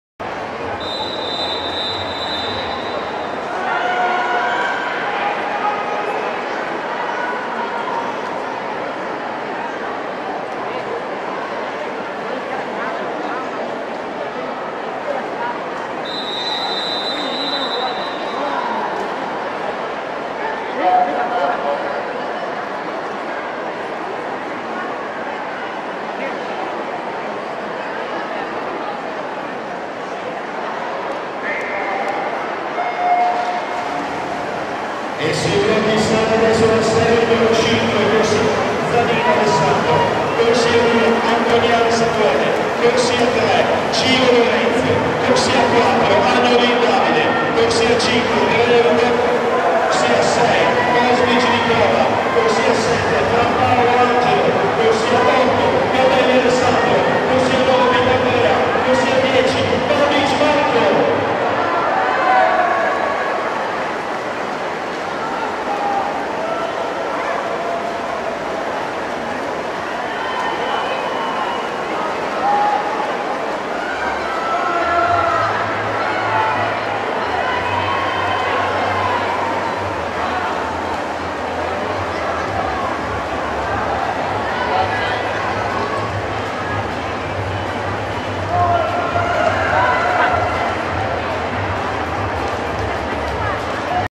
100 Stile Libero Ass. Maschile Agonisti (Serie 6) - 31 Trofeo Velocita' Soranzio - 19 Memorial Mari
Gara di nuoto svolta a TRIESTE Piscina Bruno Bianchi alle 16:16 di Sabato 01/12/2018

Atleti partecipanti:
in corsia 1 ZANETTI FRANCESCO (2002) della soc. RARI NANTES TRENTO iscritto con 00:57.33
in corsia 2 FALESCHINI LUCA (2001) della soc. ORIZZONTI iscritto con 00:57.20
in corsia 3 MORGAN ANDREA (2001) della soc. TRIESTINA NUOTO iscritto con 00:57.00
in corsia 4 BOSCHETTI GABRIELE (2003) della soc. PALLANUOTO TRIESTE iscritto con 00:57.00
in corsia 5 ROCCO GIOSUE' (2003) della soc. MONTEBELLUNA NUOTO iscritto con 00:56.97
in corsia 6 GAMBAROTTO GIULIO (2004) della soc. HYDROS iscritto con 00:57.00
in corsia 7 TOMMASSINI FEDERICO (2002) della soc. VIS SAURO NUOTO iscritto con 00:57.00
in corsia 8 FACCHIN GIOVANNI (2000) della soc. ORIZZONTI iscritto con 00:57.00
in corsia 9 BUTTARELLI LUCA (2003) della soc. A.S.D. NATATORIUM TREVISO iscritto con 00:57.29
in corsia 10 BOZ FEDERICO (1995) della soc. SAN VITO iscritto con 00:57.34